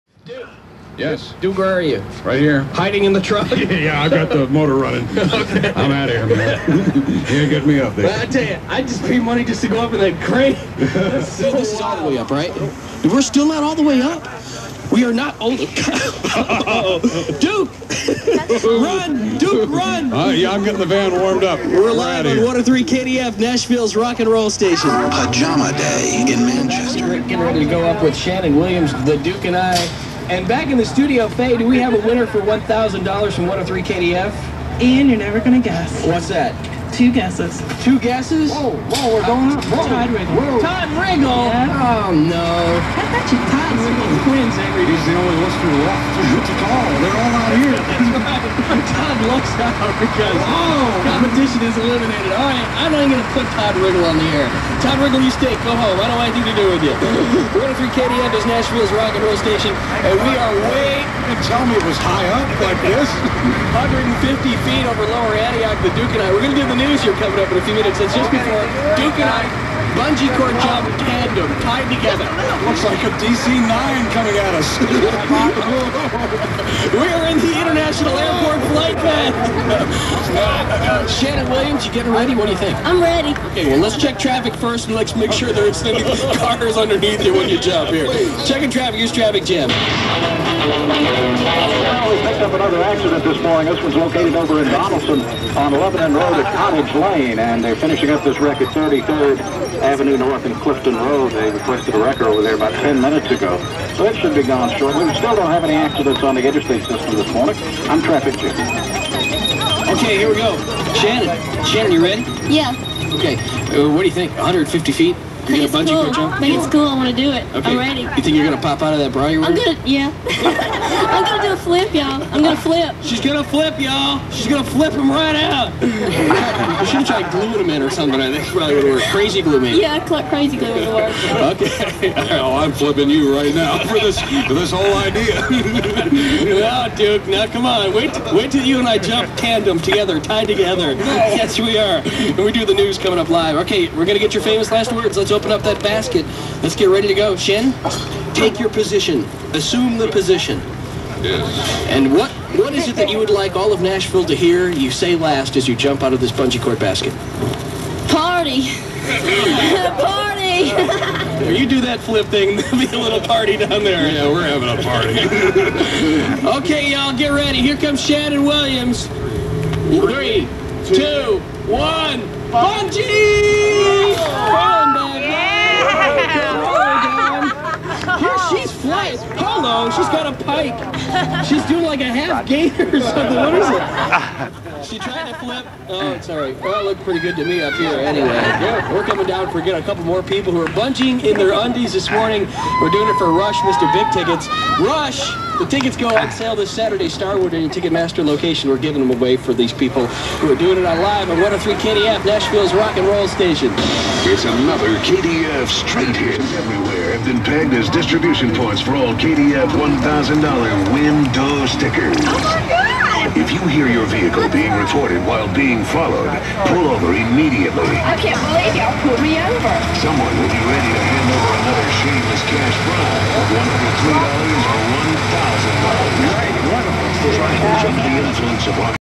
SHANNON BUNGEE'S IN HER UNDIES
Shannon will do strange things to get tickets to see Rush